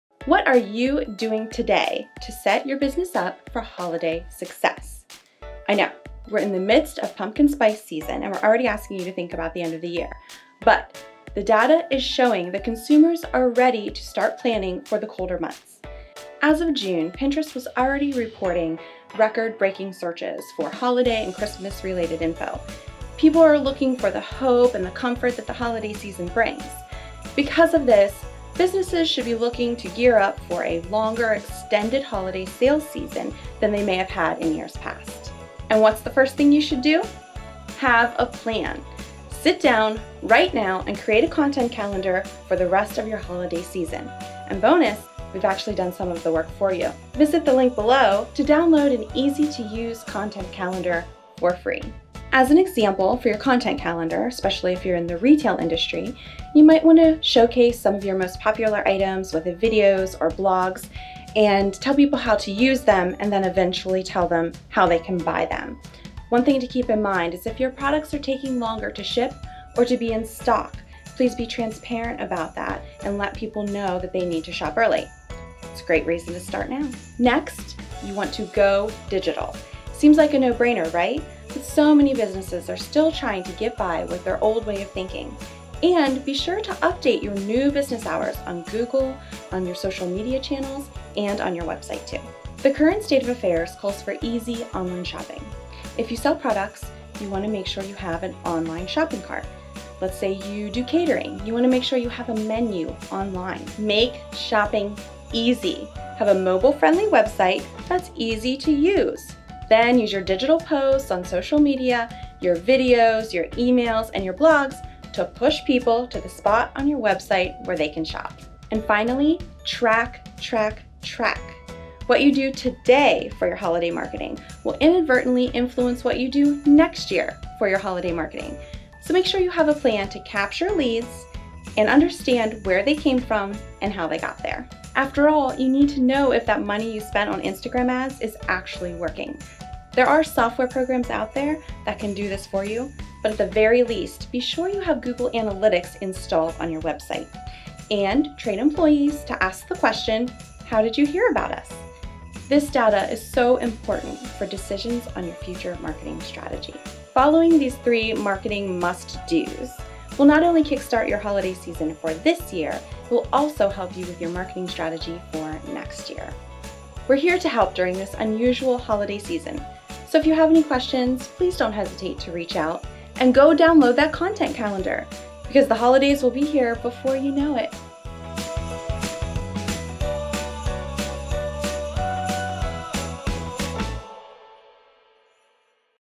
3 “Must Dos" for a Successful Holiday Marketing Campaign
The Holidays are starting earlier this year. Because people are hoping for happier days and inadvertently have more time at home, they’re ready to start decorating and gift shopping! Businesses of all sizes and in all industries should be prepared to service their clients via online shopping avenues and communications. The market expects to be able to connect with businesses easily and efficiently through just a few clicks on their phone.

If your business hasn’t yet, it’s time to transform the digital experience for your customers. Have menus available, simple shopping carts and contact forms or chatbots to field questions.

Take a few minutes to listen to some great ideas from Vanessa, our Marketing Director, to kick-start your Holiday Marketing.

Want some help? DOWNLOAD our FREE 3 MONTH CONTENT CALENDAR